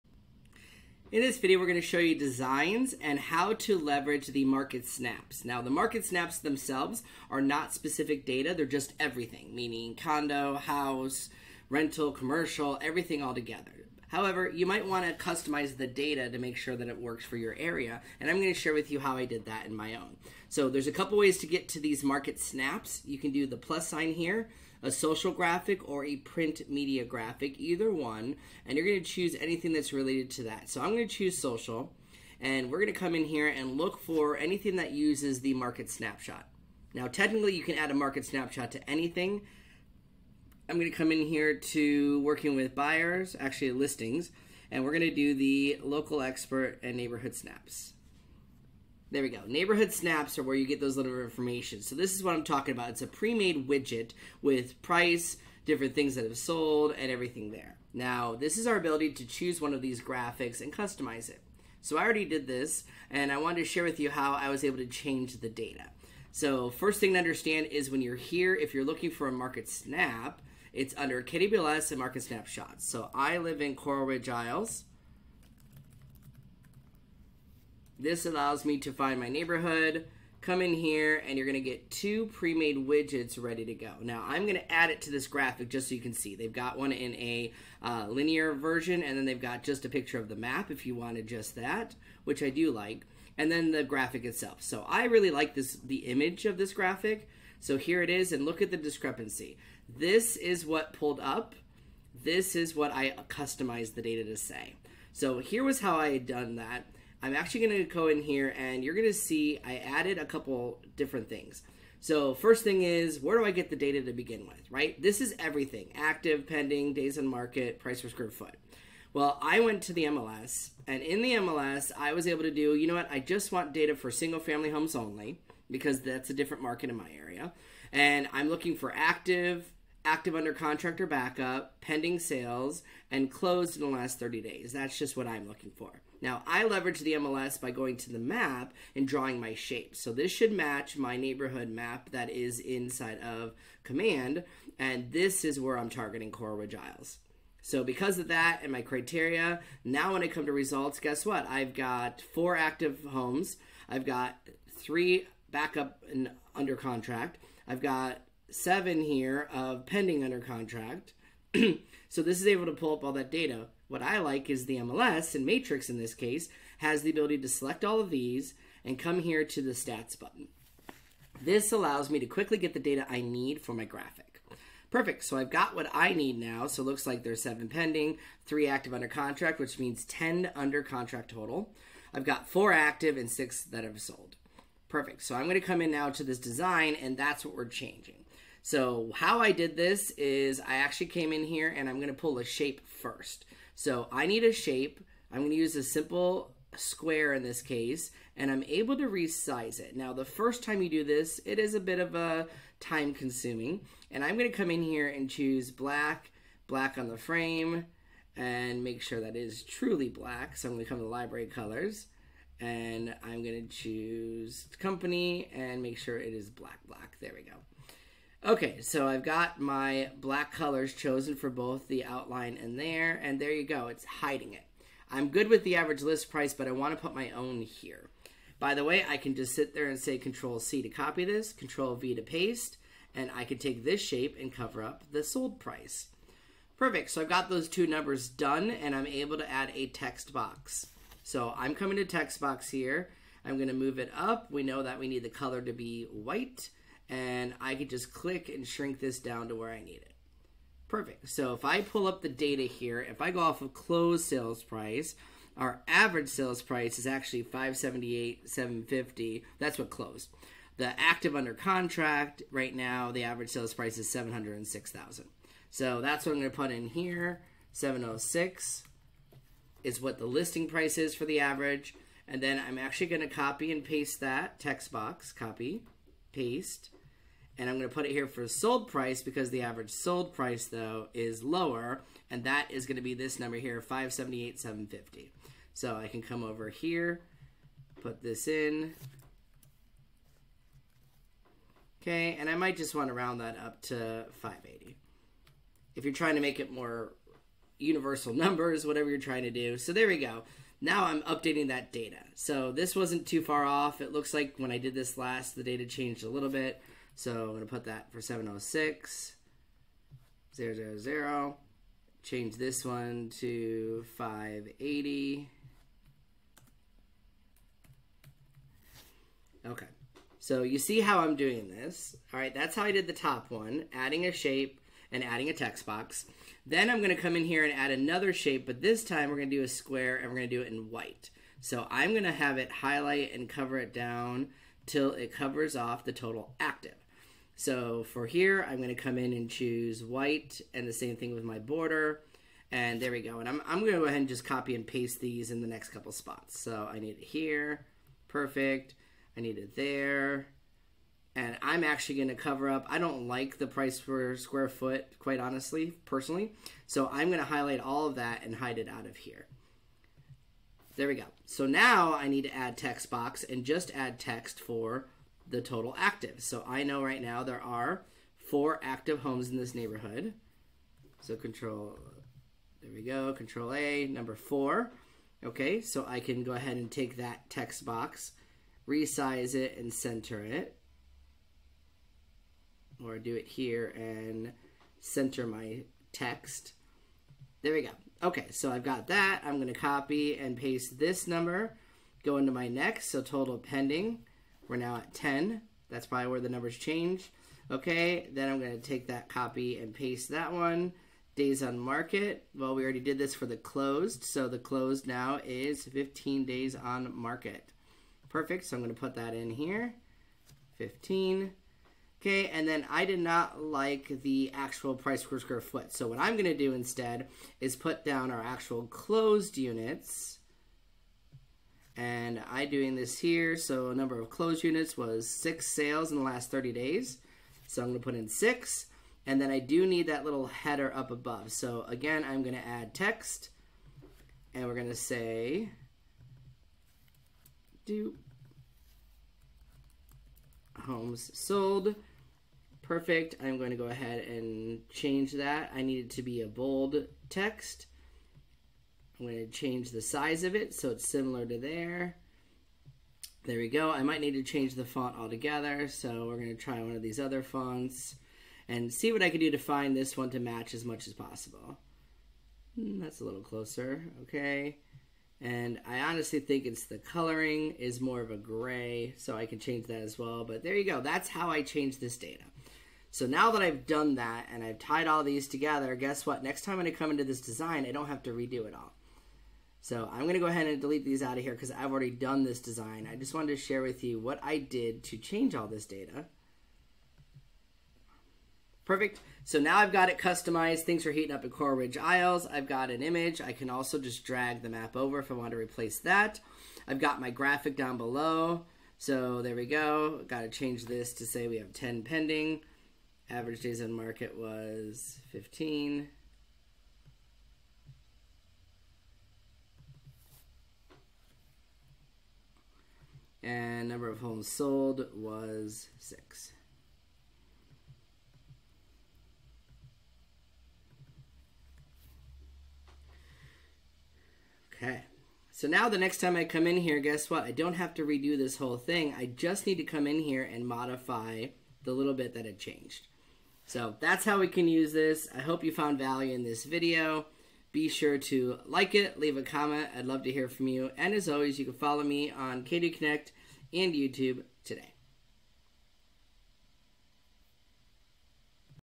KW Command: Designs - Market Snapshot Customization
In this video learn how to make Market Snap designs for Social & Print in Command and then customize the data!  Using Shapes and Text you can customize the "layout" of a snap and put your own data in!

Follow me on KW Connect today!

Keep us in Mind for any of your Fort Lauderdale and South Florida Referral Needs!